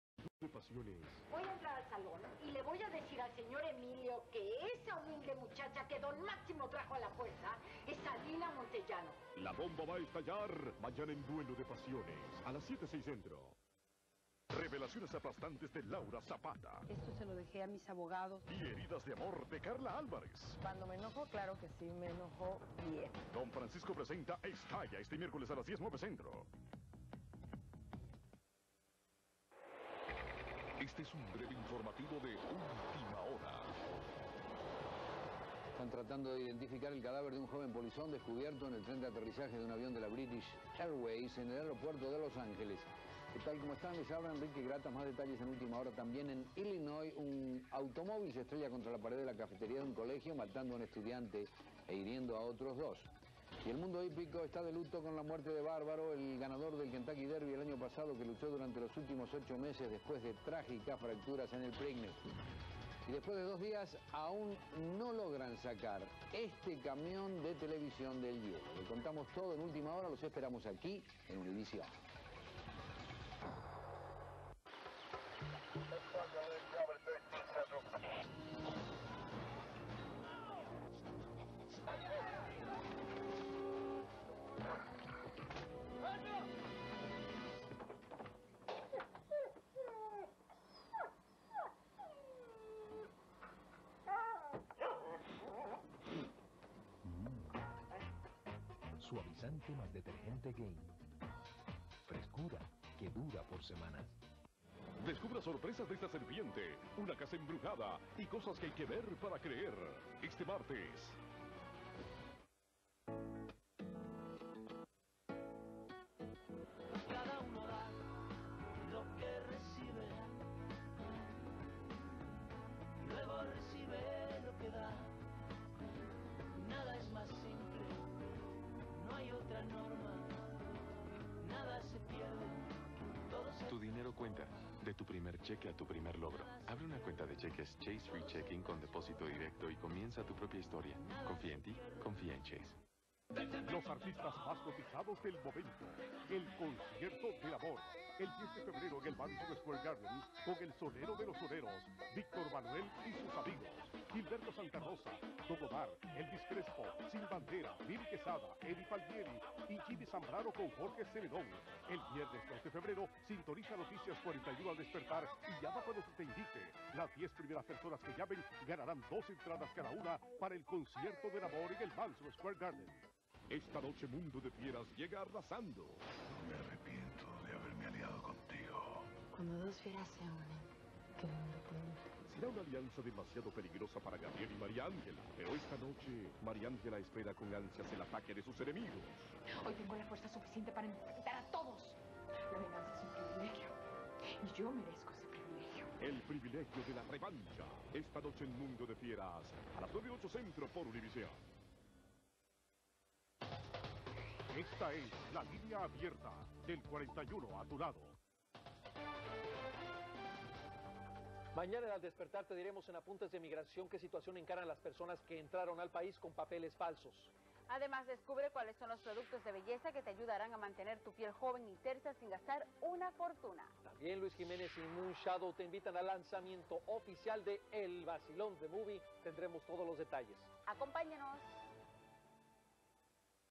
TV Hispana -Tanda de Anuncios : WXTV 41 NY - Univision (2007)
Tanda de Anuncios visto en el canal WXTV canal 41 de Univision en Nueva York. Anuncios sin fines de lucro. Perdone por la calidad del audio.